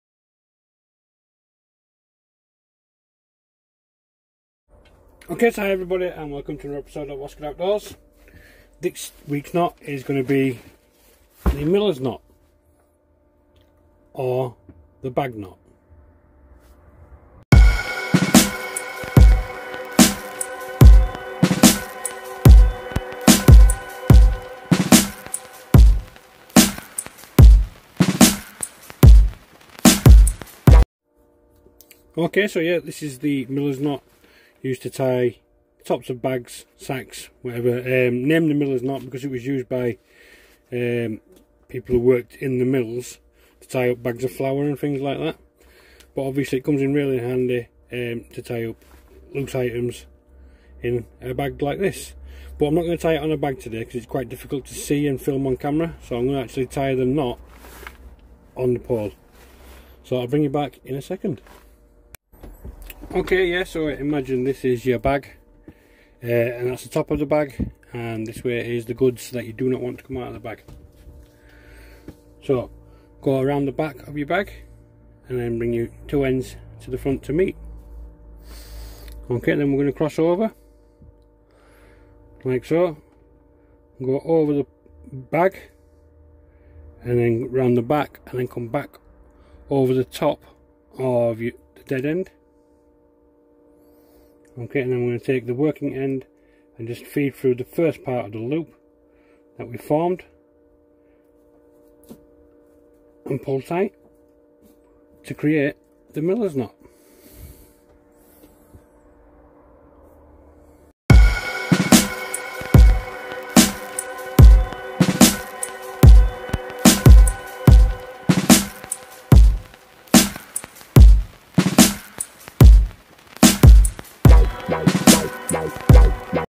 How to Tie A Millers Knot
Receiving a Book of Knots for Christmas I decided to do a series on tieing knots.

Being in Scouting I thought this would be very useful for the young members to view as well.

Scouting